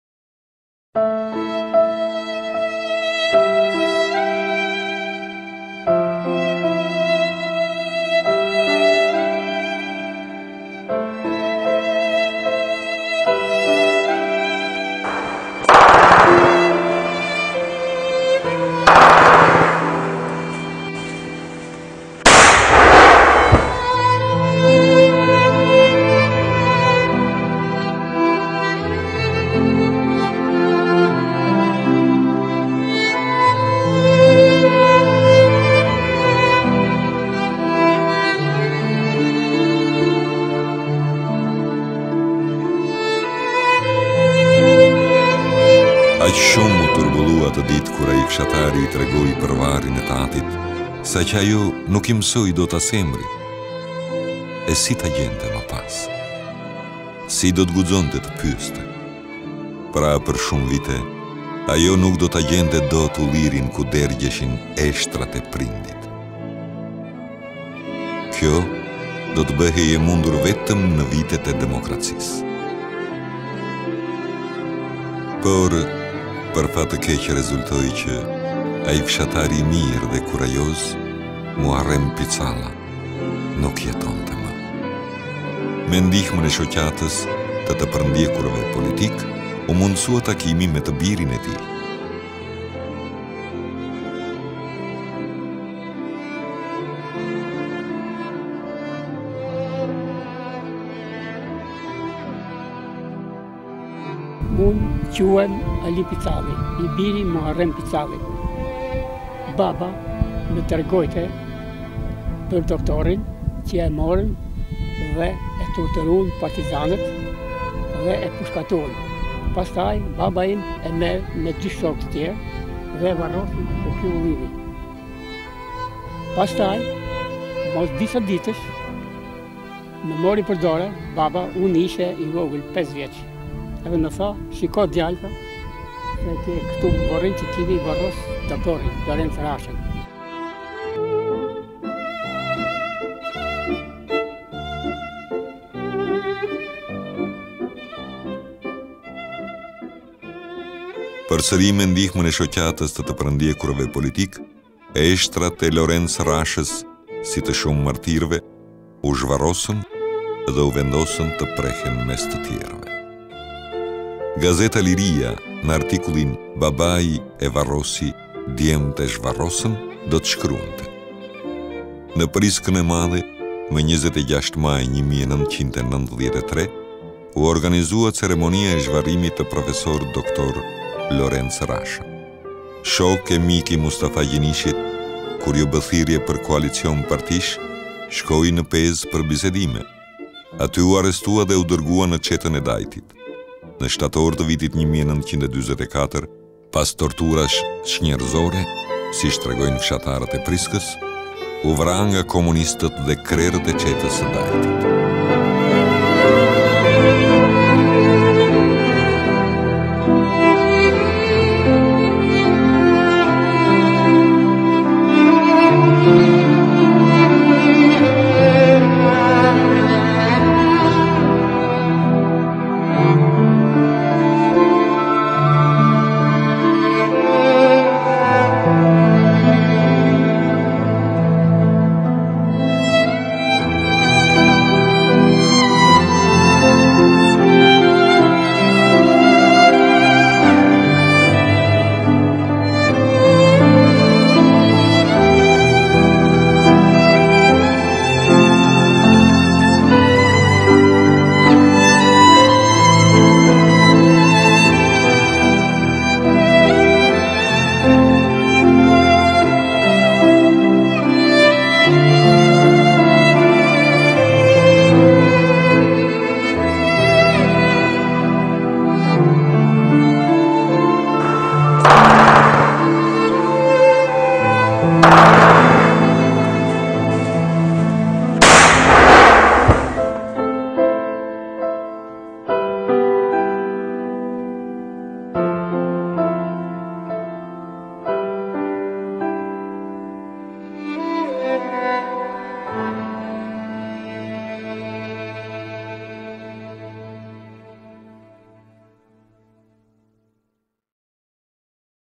LORENC RASHA - Nderi Kombit ® PANTHEON
Një prej intelektualëve e atdhetarëve të njohur të viteve 30-40, Lorenc Rasha, i lindur në Shkodër 1900, mbaroi studimet e larta për gjuhësi,me titujt Profesor Doktor, drejtor i gjimnazit të Shkodrës e Tiranës,  pas torturave mizore u ekzekutua nga komunistët e Çetës së Dajtit në shtator 1944.